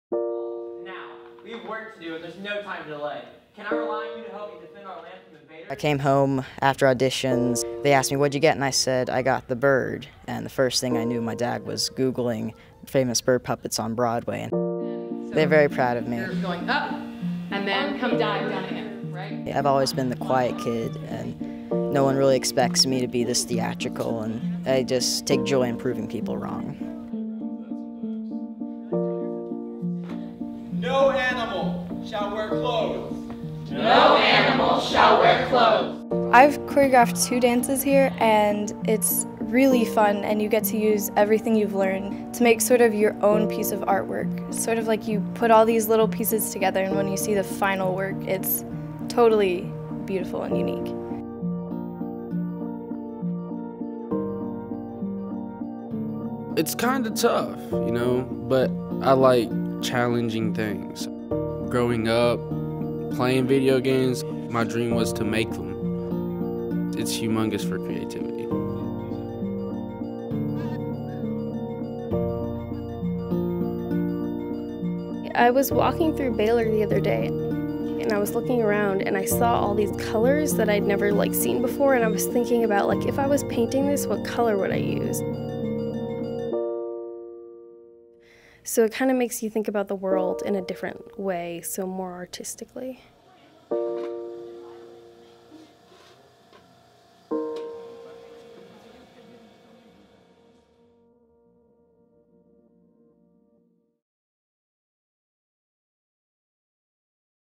The Arts at Baylor
The Arts thrive at Baylor School in Chattanooga. Theatre, chorus, band, orchestra, dance, painting, drawing, sculpture, pottery, digital design and photography are all offered at Baylor and the school calendar is packed each year with performances, concerts, exhibits, and special trips.